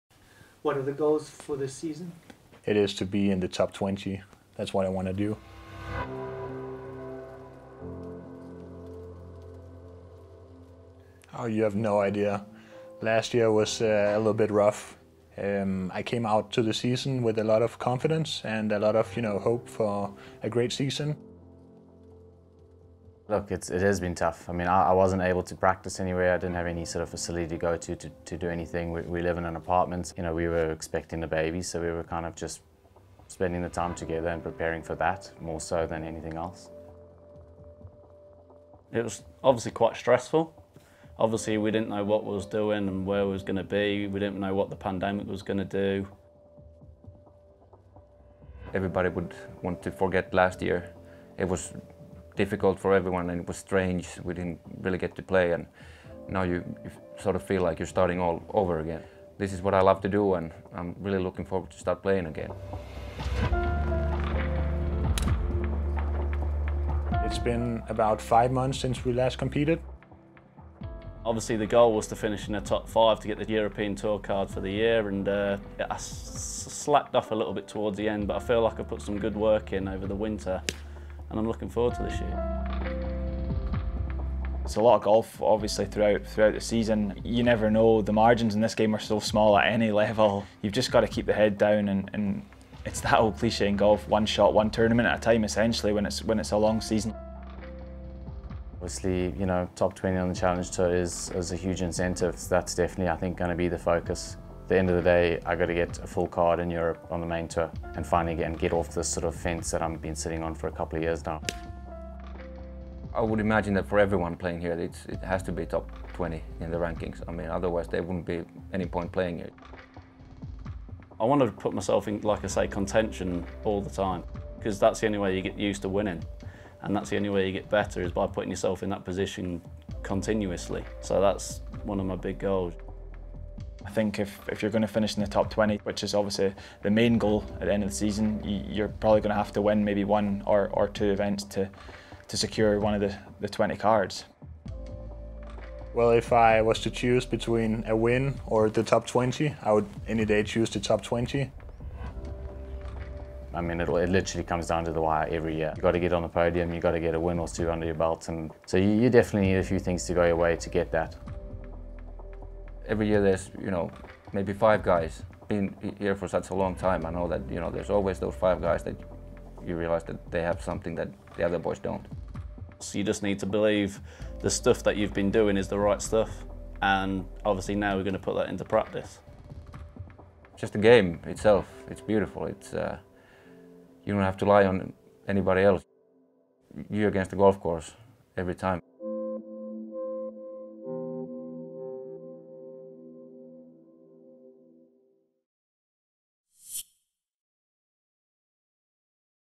2021 Challenge Tour Season Goals
2021 Challenge Tour Road To Mallorca Season Goals - Niklas Nørgaard MØLLER, Daniel Young, Bryce Easton, Jordan Wrisdale, Roope Kakko - Limpopo Championship